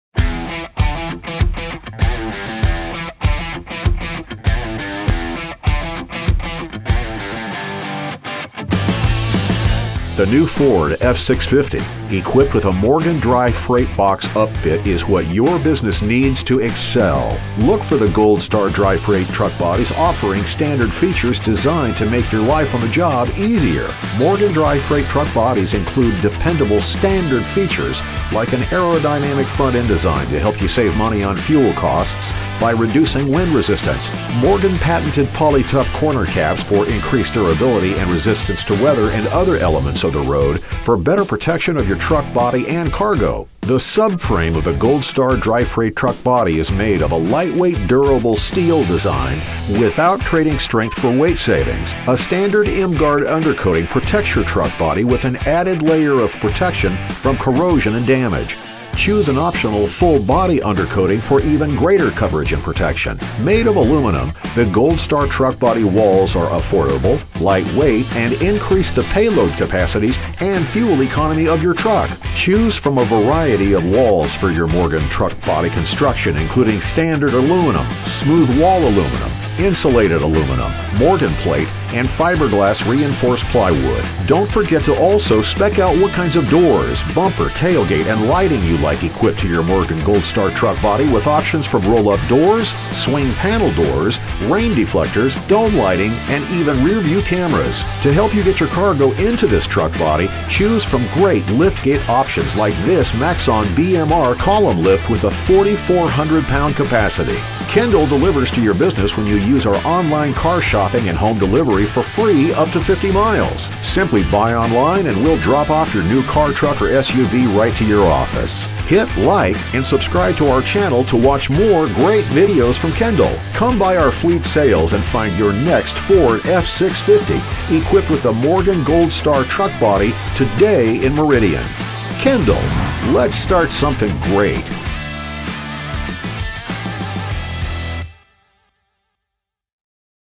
Haul Confidently with the Morgan Gold Star Dry Freight Box!
The new Ford F-650 equipped with a Morgan Dry Freight Box upfit is what your business needs to excel! Look for the Gold Star dry freight truck bodies offering standard features designed to make your life on the job easier.